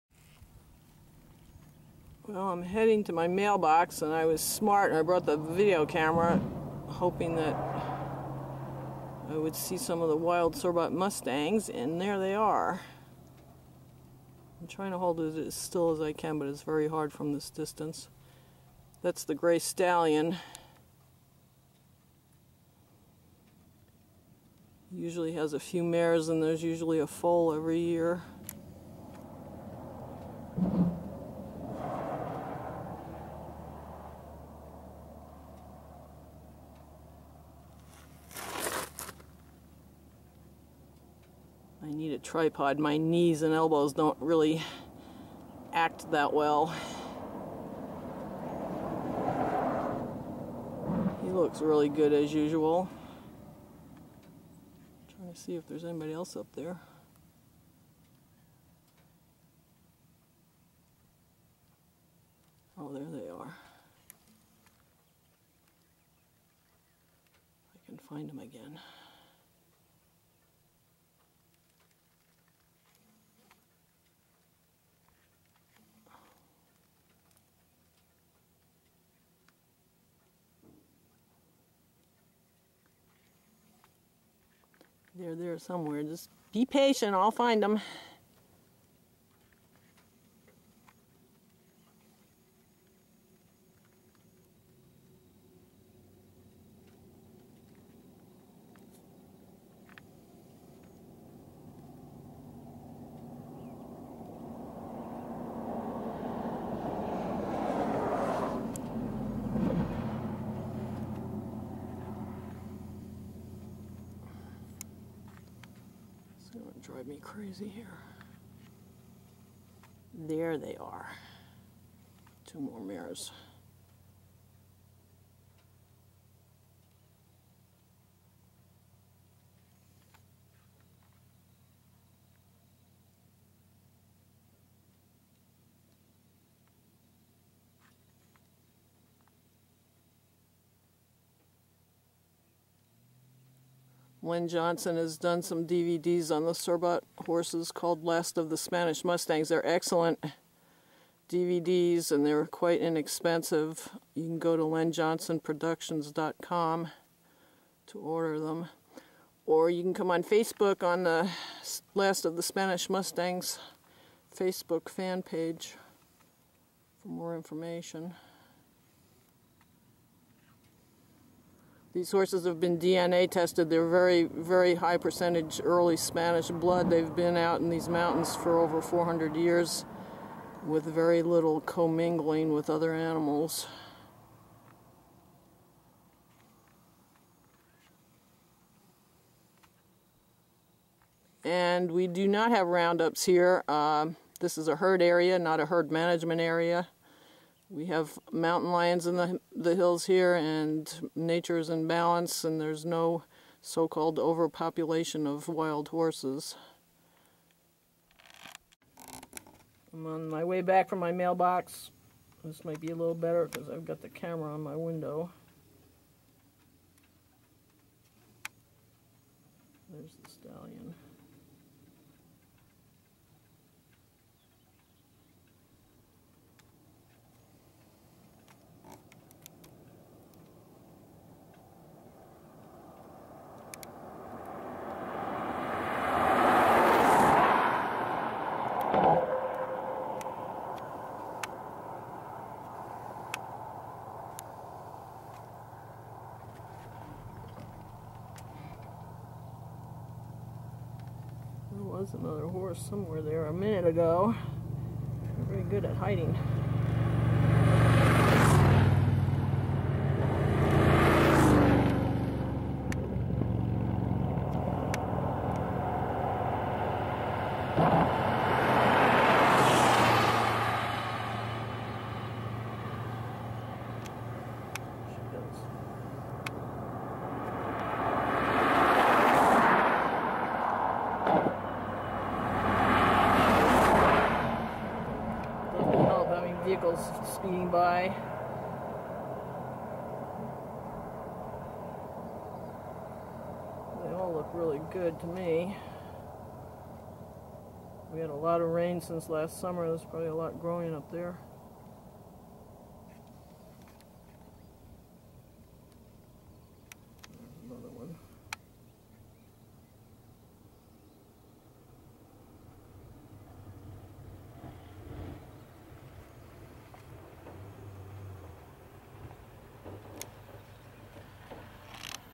Cerbat horses 3/30/15
Cerbat mountain wild horses, north of Kingman, Az. Sorry, not the greatest video but it's the best I could do from 1/2 mile away and no tripod.